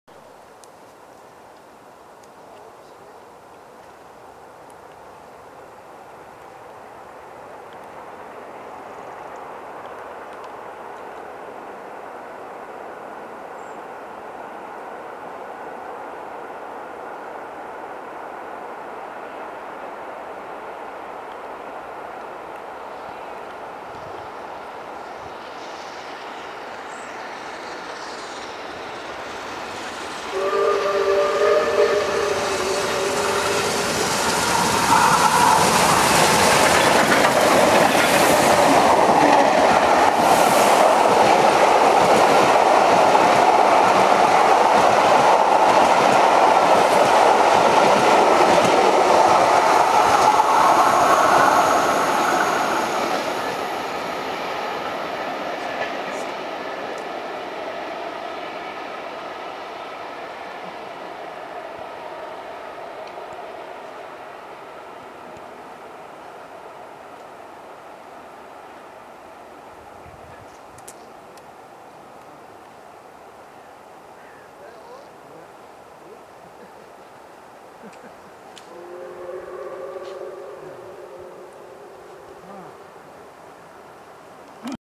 44871+45407 whistle through Freshford heading for the Bath Christmas Market 07-12-13
Yet another video of Ian Riley's Black Fives in action. This time heading the last steam railtour to Bath and Bristol before next year in 2014.

I decided to head to Freshford and  despite occasional drizzle whilst waiting for them, it was virtually dry when they finally came. They came through a couple of mins late but with a chime whistle with 44871 (how wrong is that) made it a really good shot indeed, plus the odd whistle again near the end of my video.